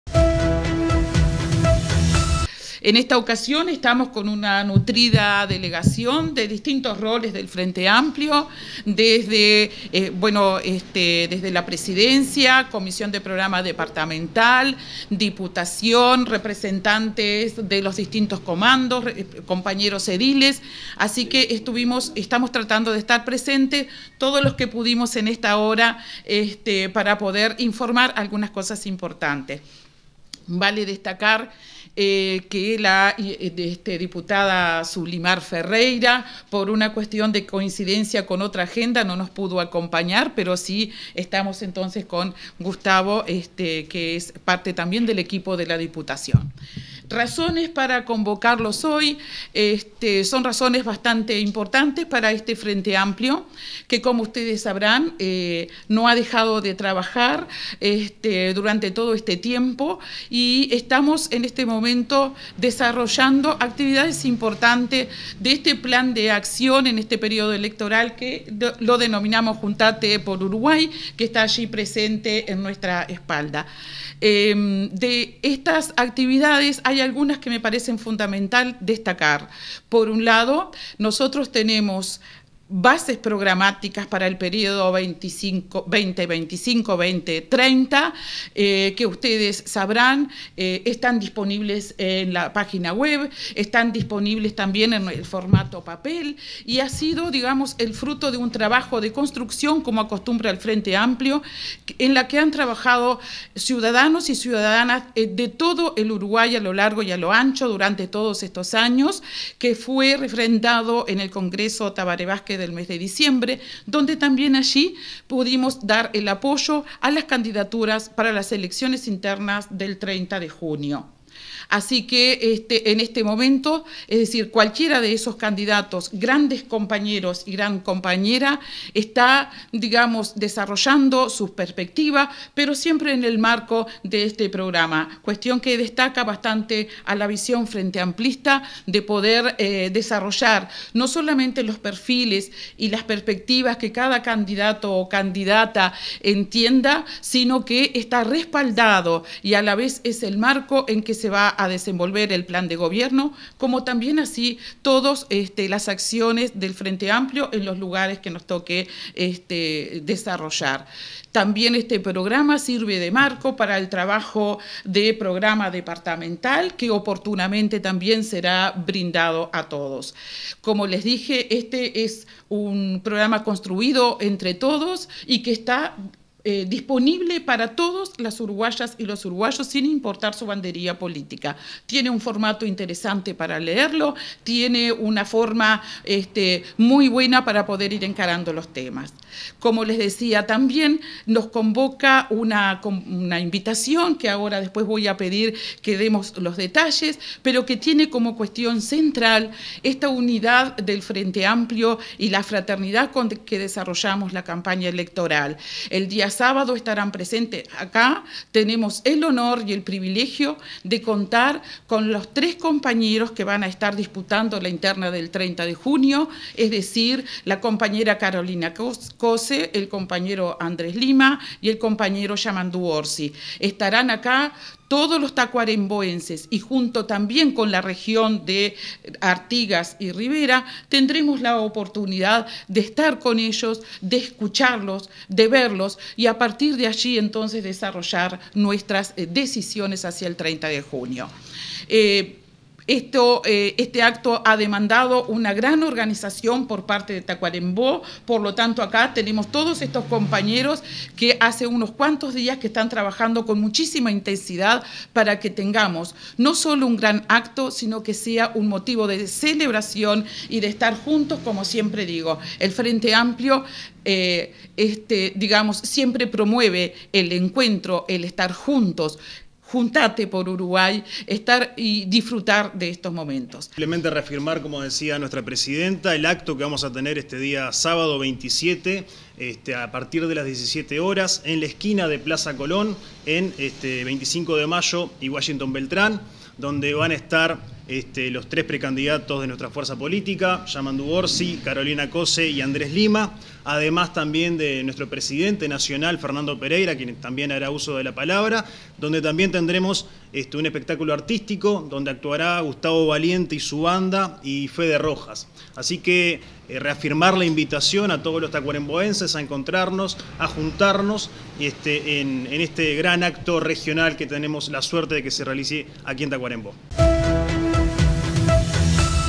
Yamandú Orsi, Carolina Cosse y Andrés Lima se presentarán en Tacuarembó
Será en un acto que se desarrollará este sábado, a las 17:30 en Plaza Colón.